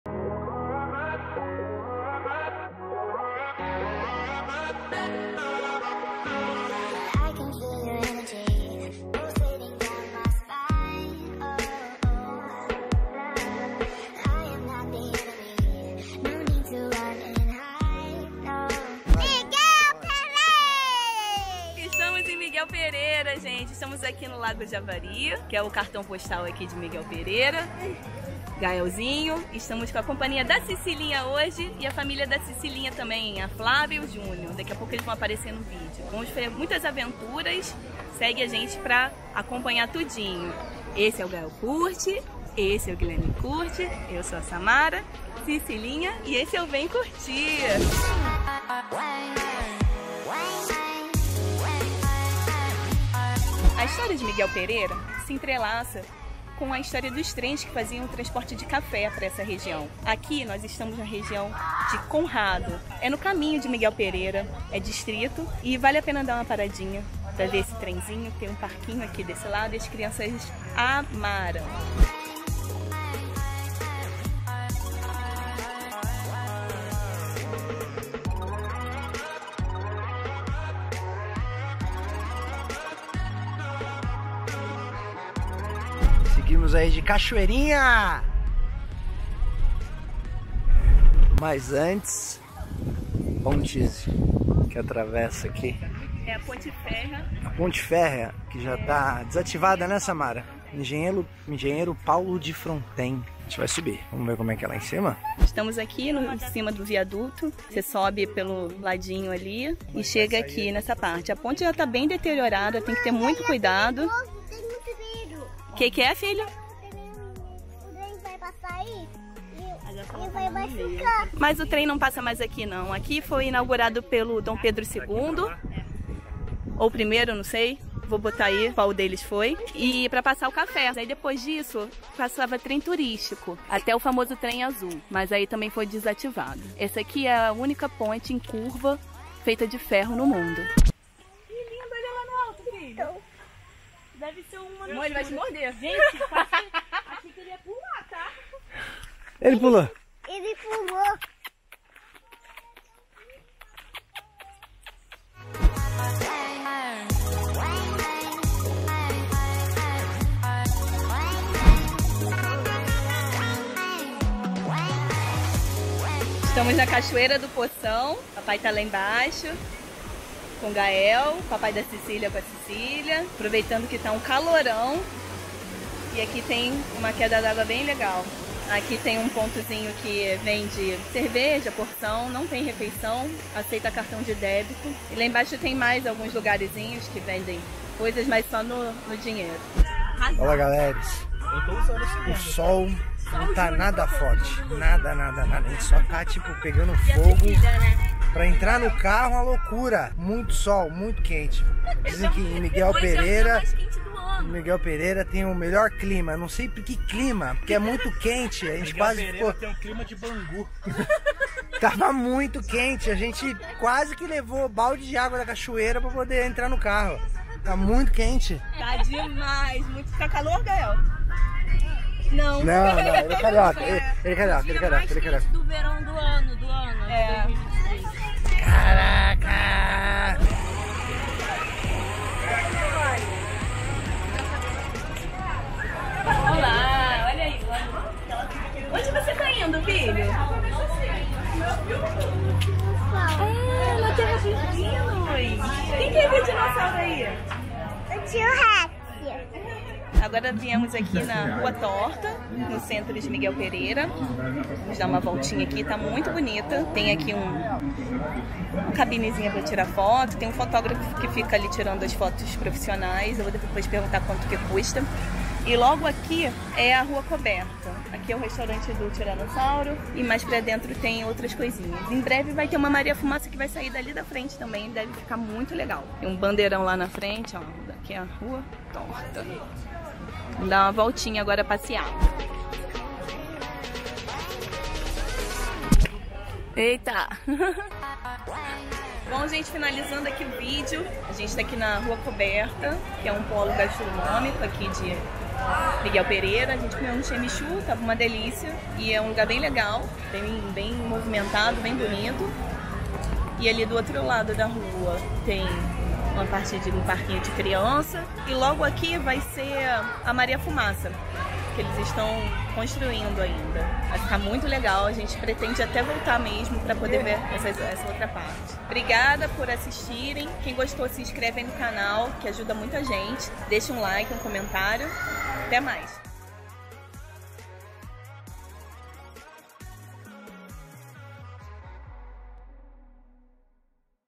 Bate e volta para Miguel Pereira RJ - Dica de passeio com crianças
Fizemos um Bate e volta para Miguel Pereira RJ, que é um município brasileiro do estado do Rio de Janeiro, localizado no centro-sul fluminense. As crianças queriam ir na recém inaugurada Terra dos Dinos, que teremos um vídeo completinho aqui no canal e aproveitamos para passear por essa cidade que tem investido bastante em turismo e já conta com novidades muito legais para passar bons momentos com a família.

Aqui Guilherme Curty, Gael Curty, Samara e esse é o "Vem Curty"